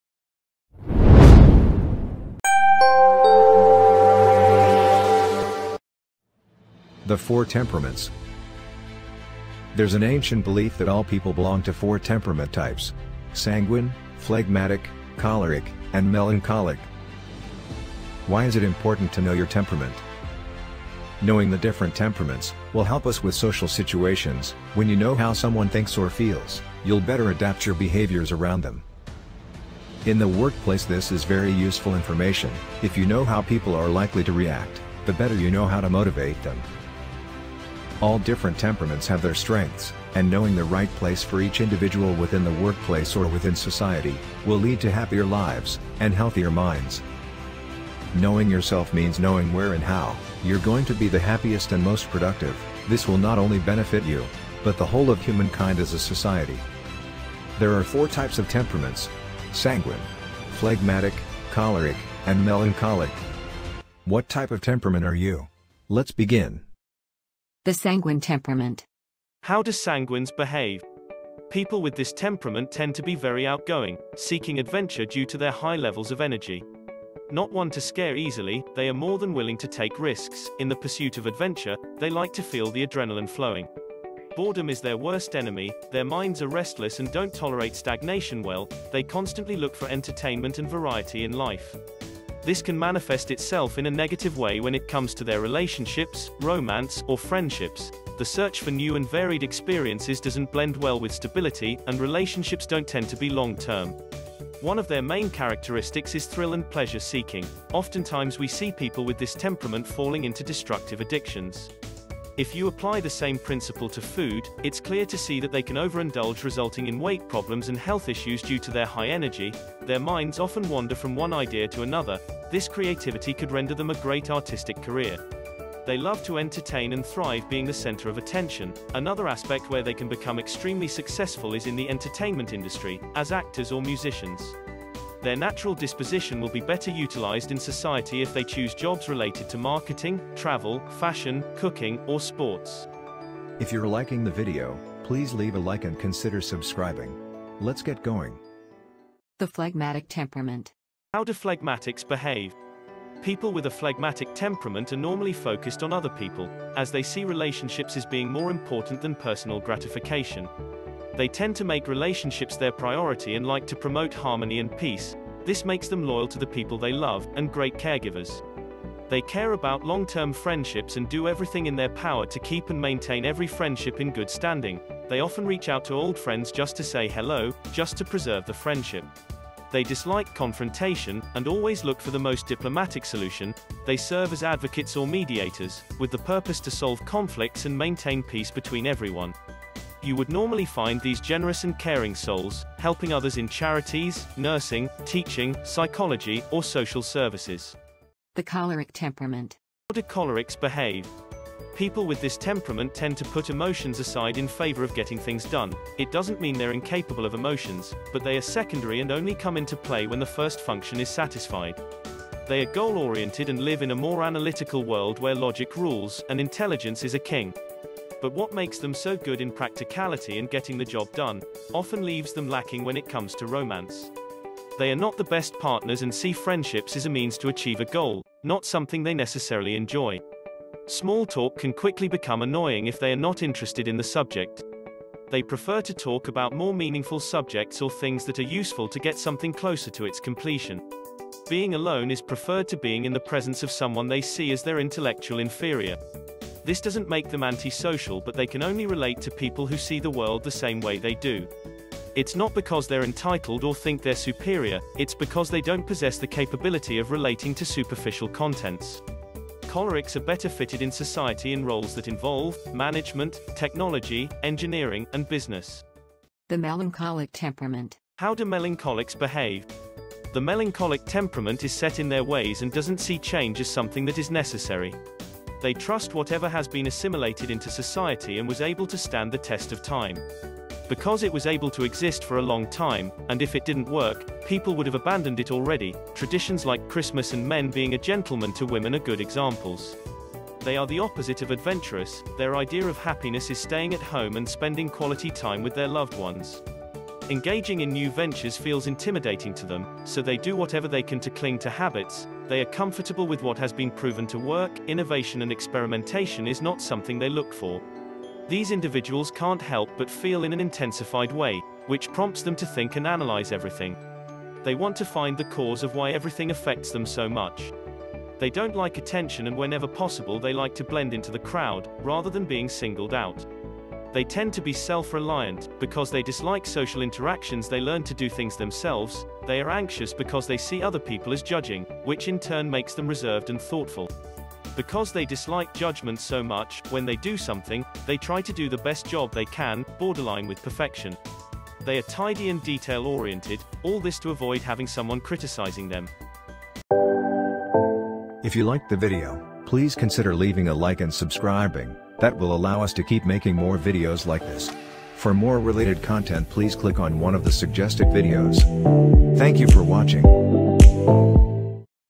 4 Temperaments (Which One of The 4 Temperaments Are You? )
In this video, we cover the 4 Temperaments and why It’s important to know yours.
Knowing the different temperaments will help us with social situations, when you know how someone thinks or feels you’ll better adapt your behaviors around them. You’ll be able to accurately predict how someone will react and, avoid creating uncomfortable or combative situations.